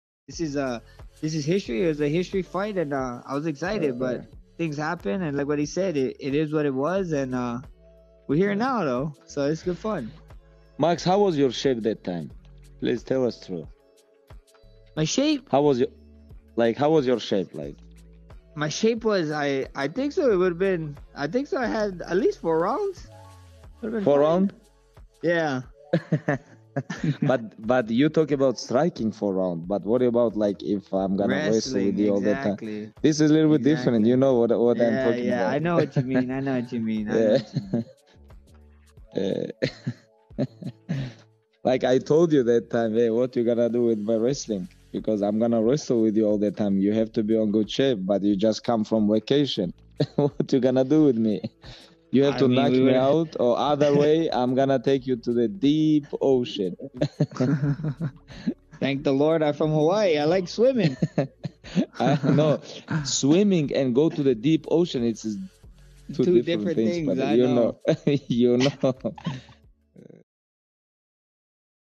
Хабиб Нурмагомедов и Макс Холлоуэй обсудили, что могло бы произойти в их несостоявшемся бой
⚡️🌍 Пресс-конференция в метавселенной. Вчера Хабиб и Макс Холлоуэй обсудили, что могло бы произойти в их несостоявшемся бою в 2018 году 🌊😄